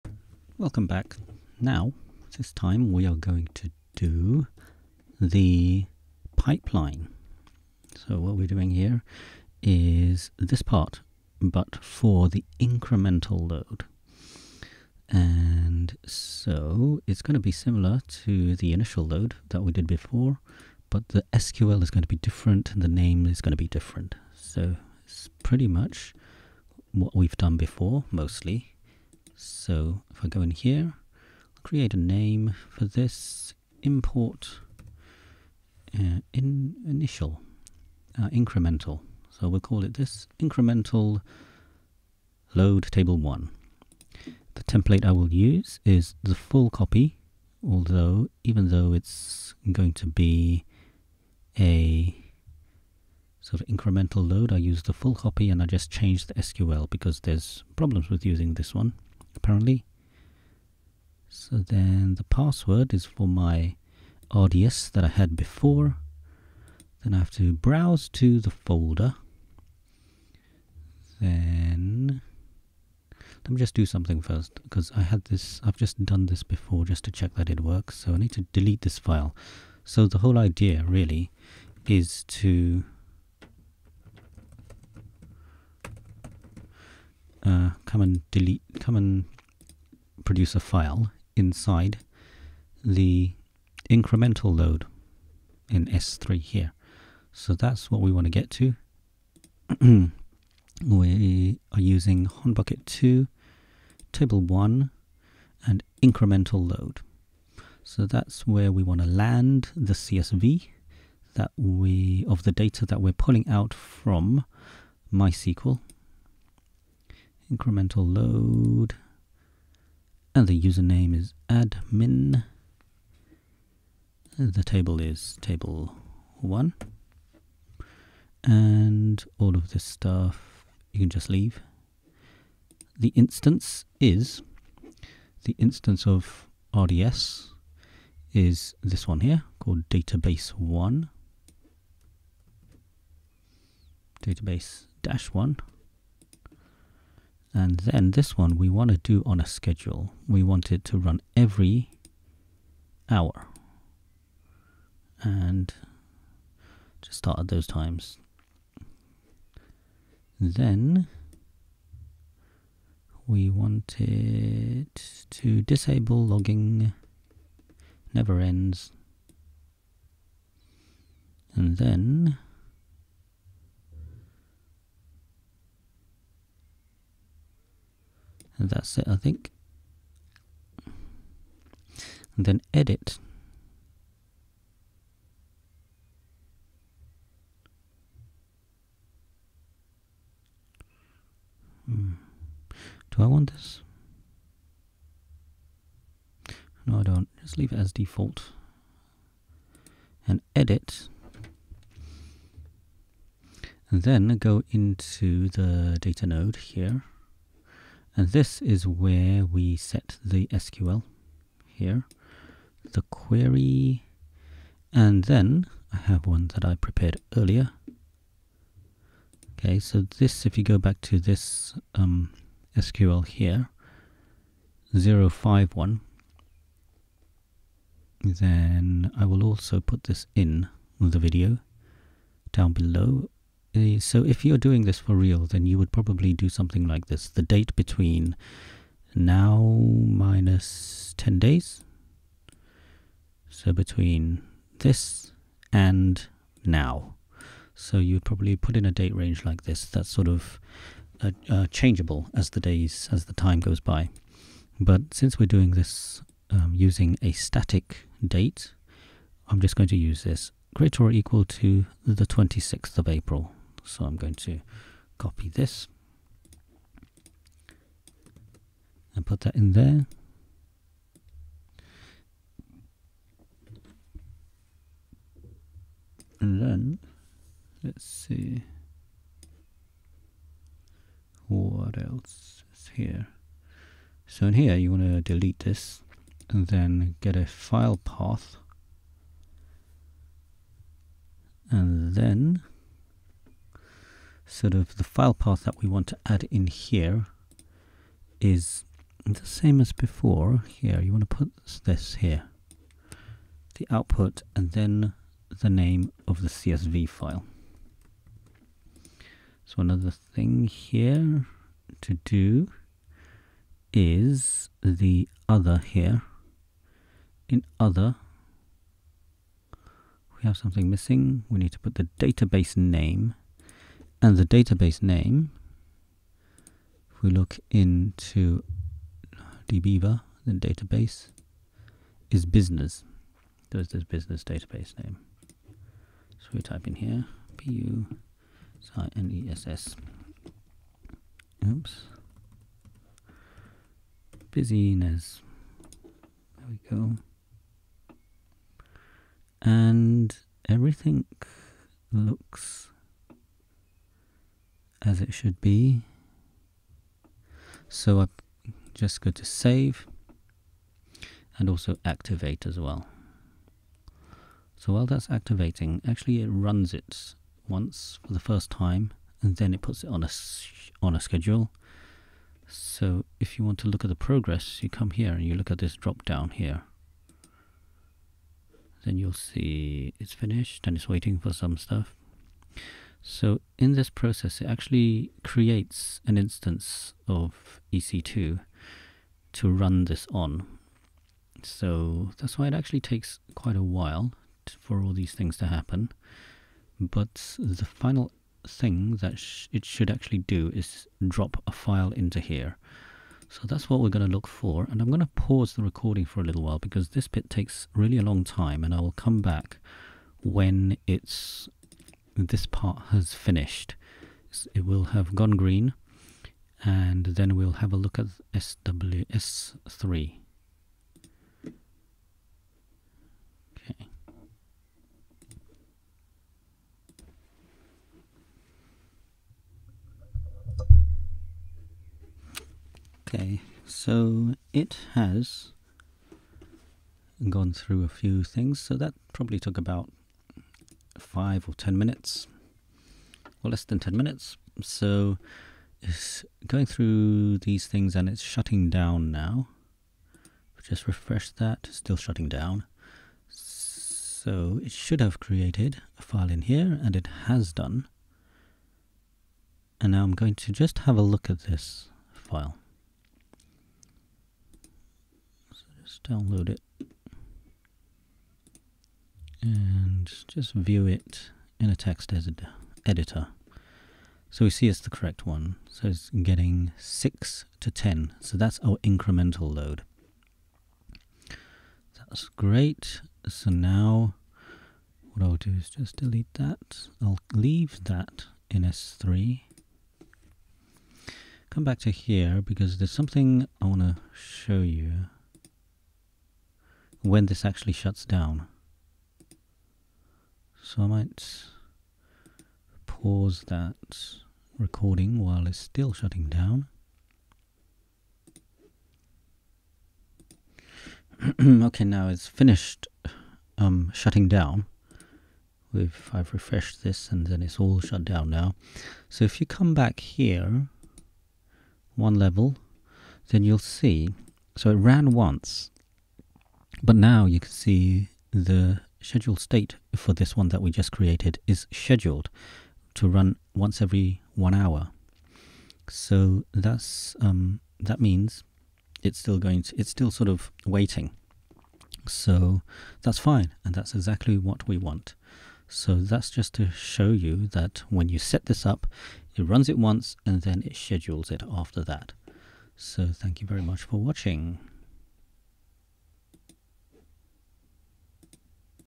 08 AWS ETL - Configure Incremental Data Pipeline from MySQL to S3 (code used below)
Code used in this video...